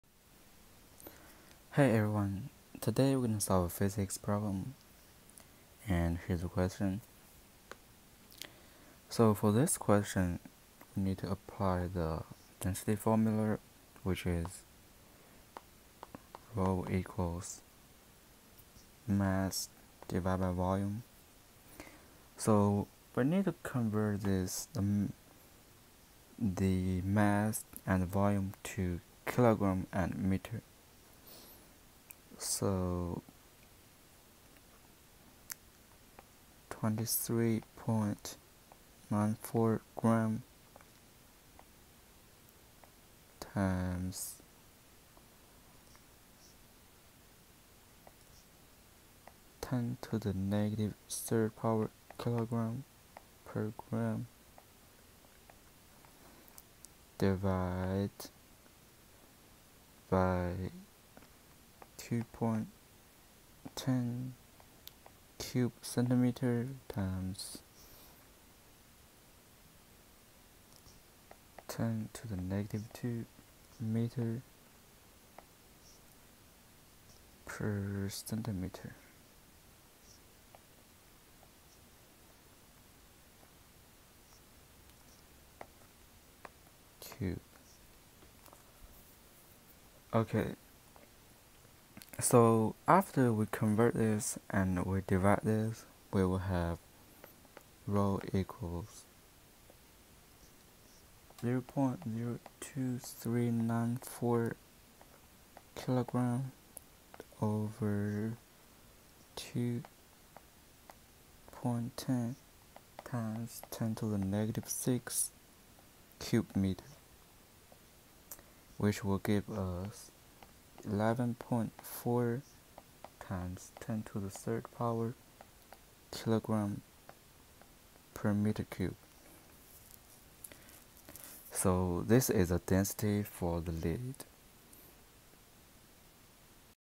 Physics for scientists and engineers, chapter 1, physics and measurement
Chapter 1, physics and measurement

15, A solid piece of lead has a mass of 23.94 g and a volume  of 2.10 cm3. From these data, calculate the density of  lead in SI units (kilograms per cubic meter).